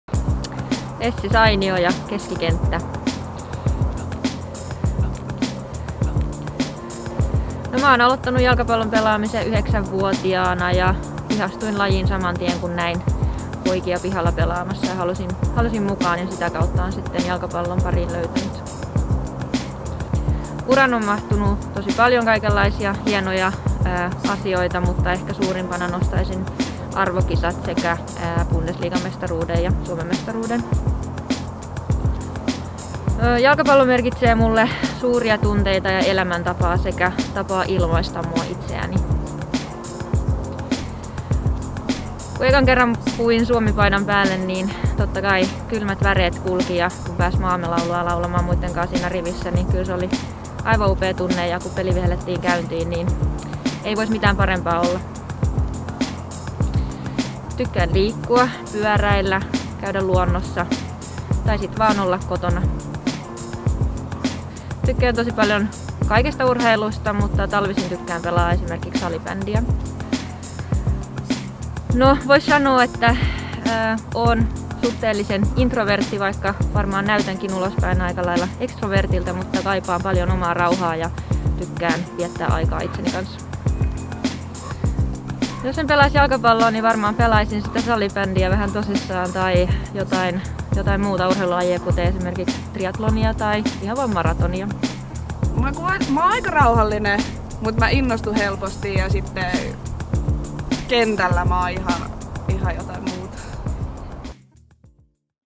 Helmarit | Viikon pelaajakortti – Essi Sainio! 🦉
Helmareiden Viikon pelaajakortissa tutustutaan joukkueen keskikenttäpelaajaan Essi Sainioon! 👋💬

🦉 Essi Sainio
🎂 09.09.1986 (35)
⚽ Keskikenttäpelaaja
🛡📌 HJK
🛡🇫🇮 FC Kasiysi, FC Espoo, MPS, FC Honka
🗣 "Jalkapallo merkitsee mulle suuria tunteita ja elämäntapaa sekä tapaa ilmaista itseäni."

Mitä mieltä olit tästä videosta? Seuraavassa pelaajakortissa ääneen pääsee Helmareiden keskikenttäpelaaja Eveliina Summanen!